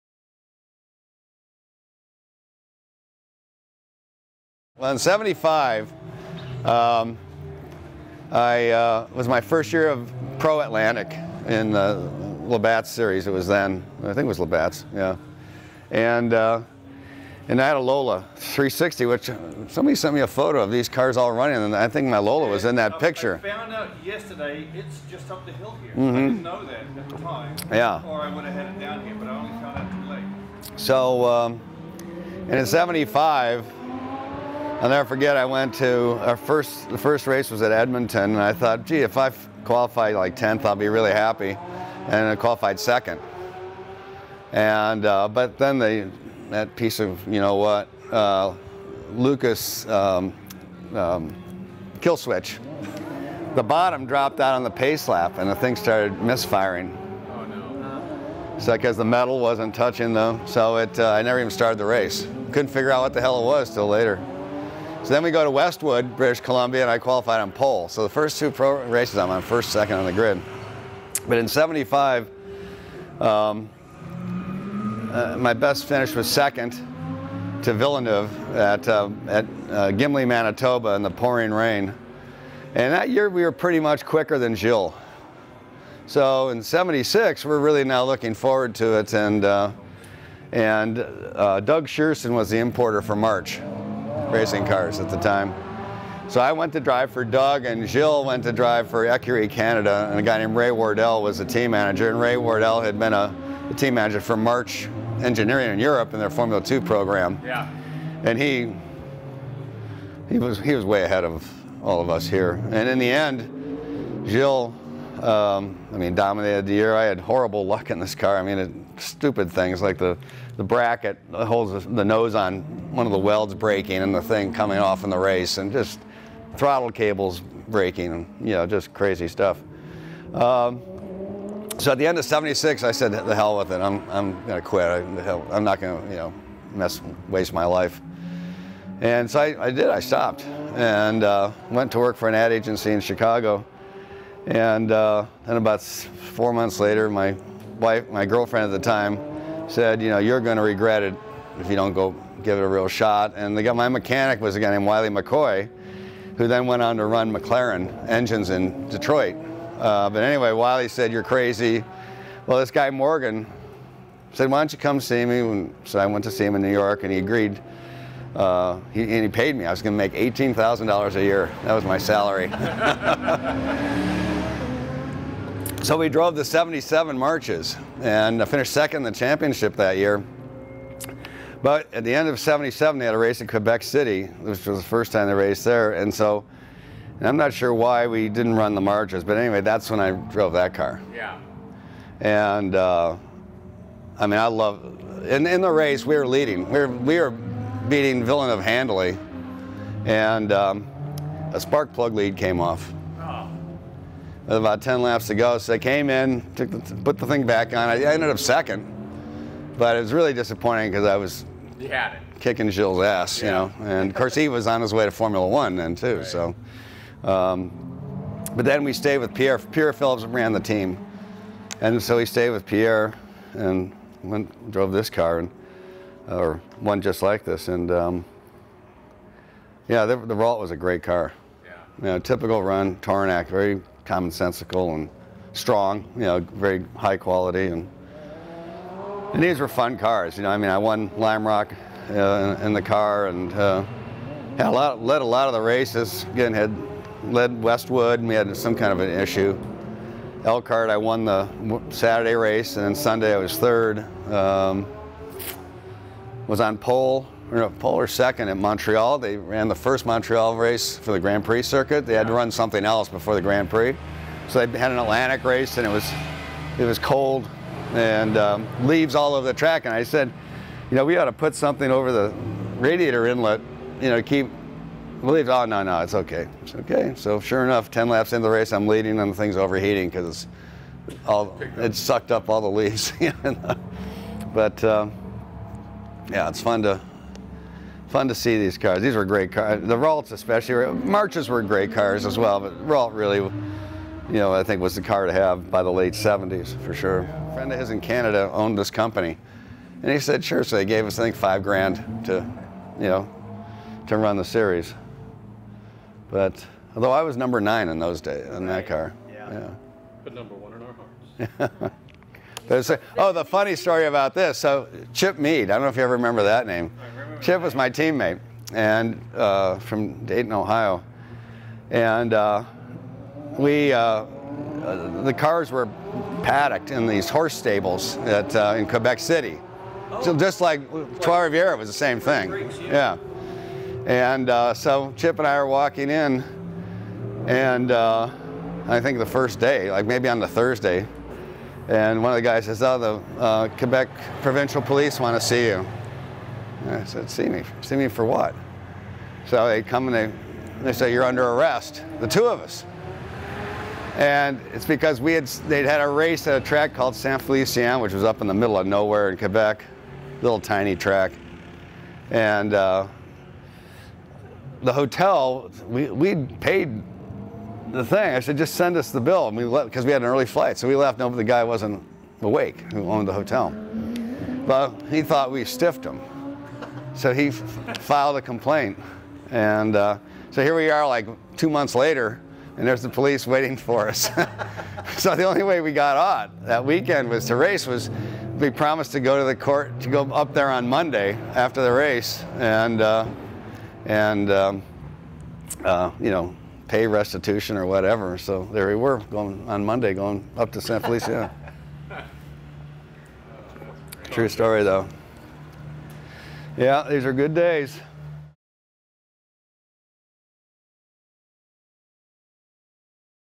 RACER: Rahal Reunited with Formula Atlantics
Bobby Rahal is reunited with three of the Formula Atlantic cars he drove in the 1970s while visiting Sonoma Raceway for the 2017 IndyCar season finale, and reveals the story of the time he was arrested before an Atlantic race in Canada...